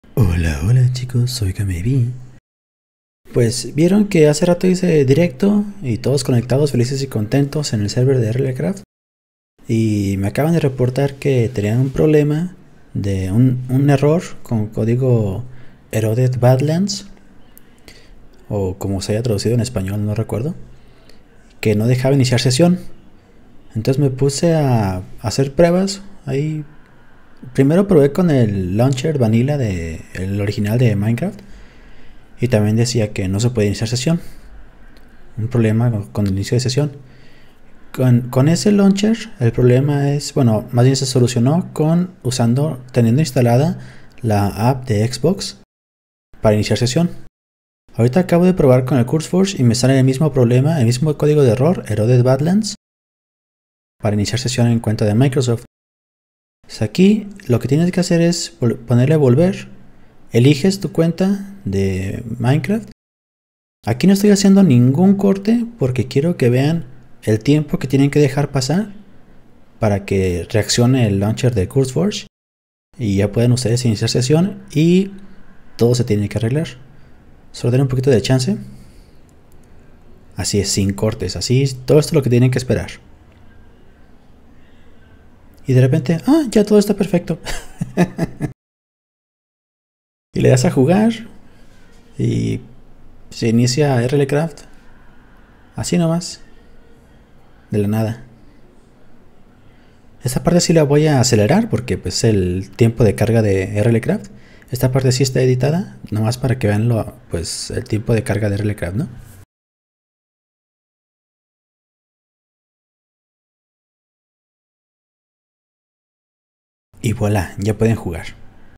SOLUCIONADO! Error "Eroded Badlands" en Minecraft y RLCraft
Aquí les comparto lo que me funcionó A MÍ para solucionarlo.
Si seguir estos pasos no les ayudó, leí que pueden probar:
1. Reiniciando la PC
2. Desinstalando el Launcher de Minecraft y reinstalándolo.
3. Simplemente esperar, ya que podría ser un problema con los servidores de XBox.

Otra cosa que está fuera de nuestro control, son los servicios de Xbox (necesarios para iniciar sesión):
Si los servicios bajo la sección "Cuenta y perfil" están caídos, sólo queda esperar.

*Este video fue grabado y publicado en tiempo récord.*
Ayúdame a crear contenido a tiempo completo!

Platica conmigo!

Juega RLCraft como yo!